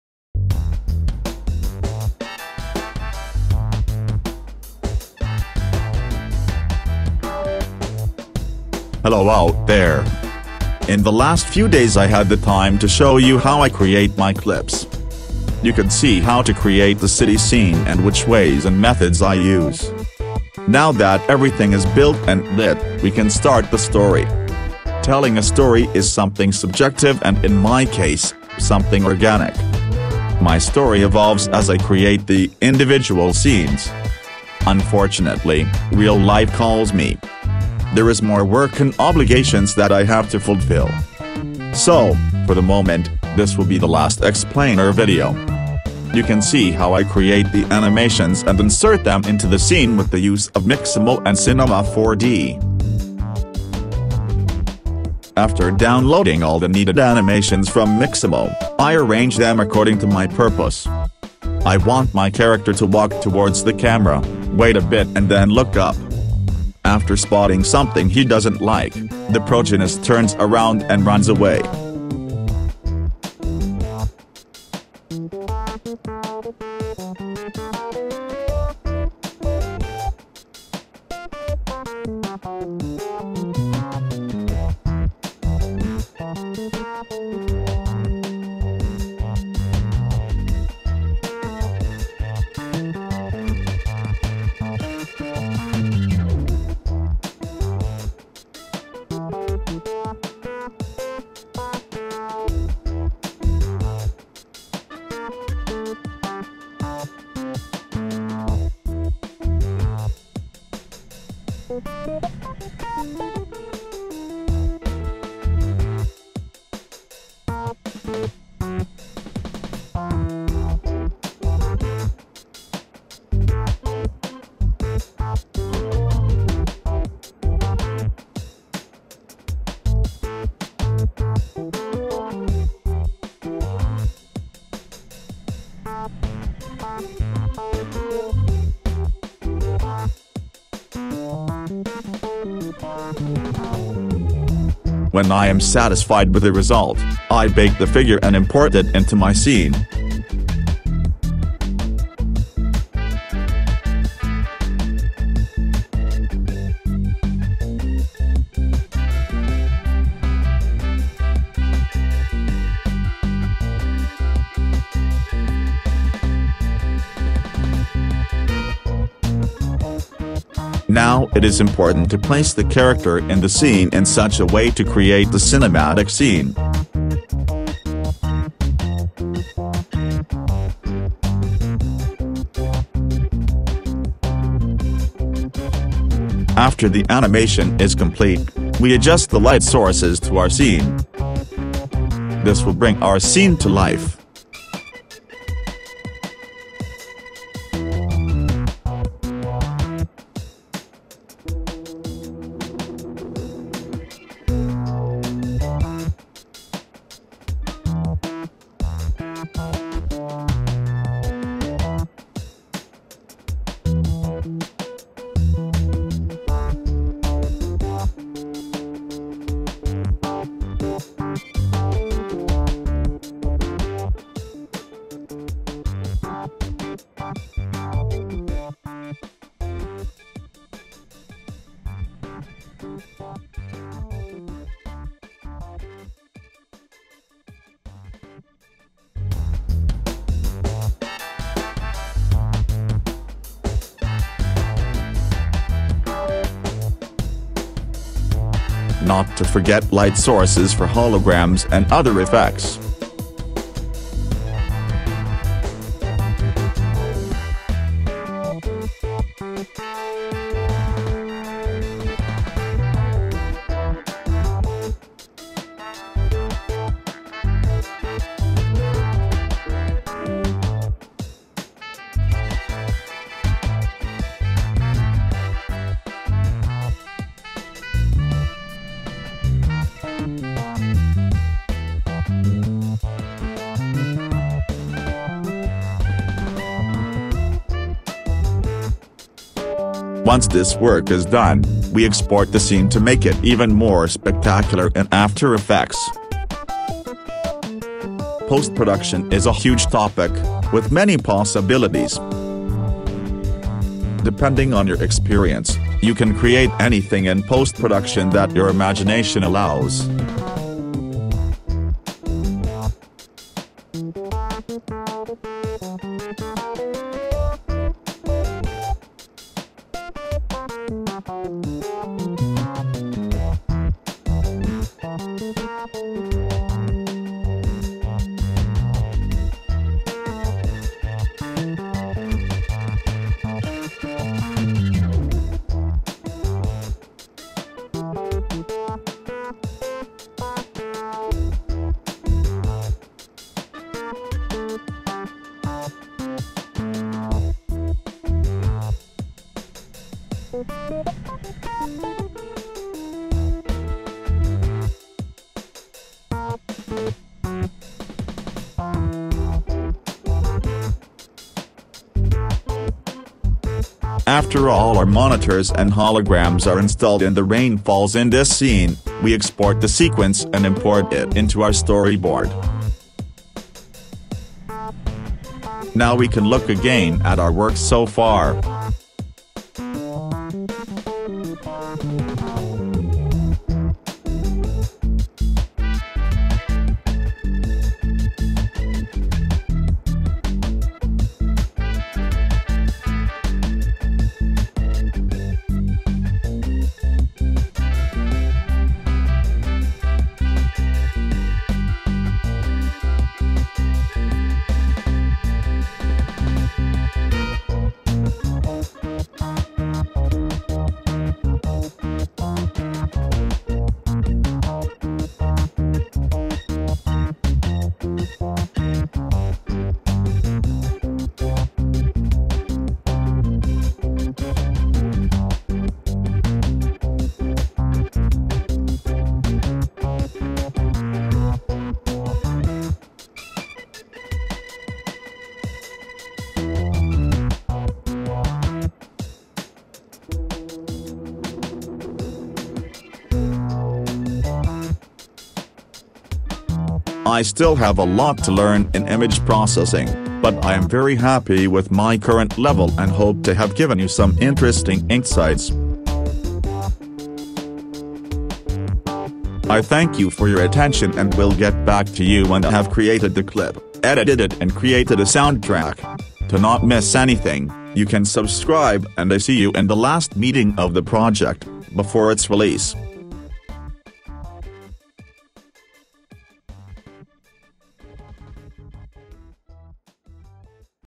CYBERPUNK / Let's make a Movie Part 09 / Storytelling
hello out there.
in the next weeks i will give you the opportunity to watch me working on the computer.
i'm creating a new film in cyberpunk style and i'll show you in the videos how i'm creating the different scenes.
pay attention and have fun.

Idea and Realization by:
Opa Grampz / Old Fart Stories

Created with:
Anima / Cinema 4d / After Effects / Premiere Pro / Audition

Titel:
Just Nasty